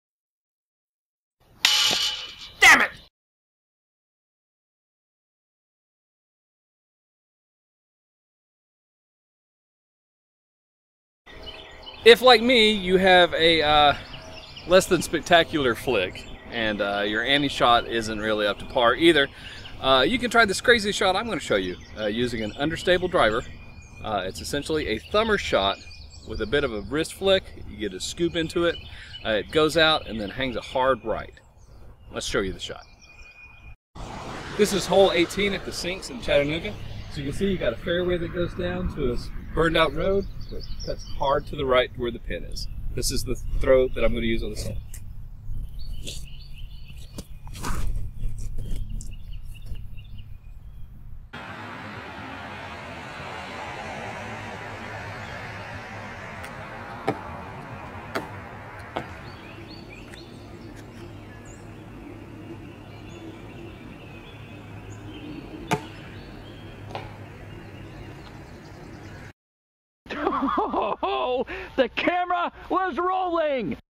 Monarch Flip
One of the Disc Golf Monkeys disc golf tips and tricks series, see how to throw a Monarch so it makes a sudden right turn in the air, without having to master a difficult anny throw.